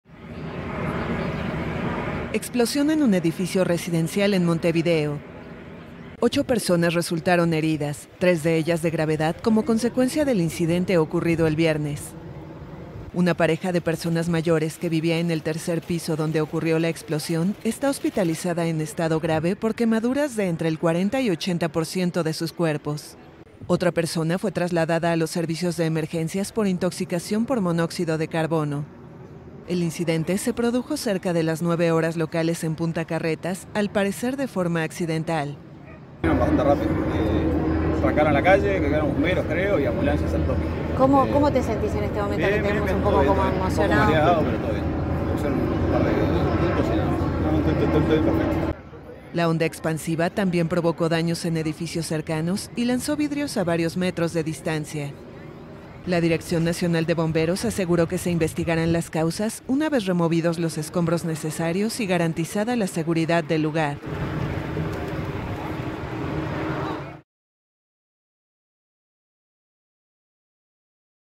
Ocho heridos tras explosión en edificio de barrio residencial de Montevideo | AFP
Ocho personas resultaron heridas, tres de ellas de gravedad, como consecuencia de una explosión el viernes en un edificio de un barrio residencial de Montevideo.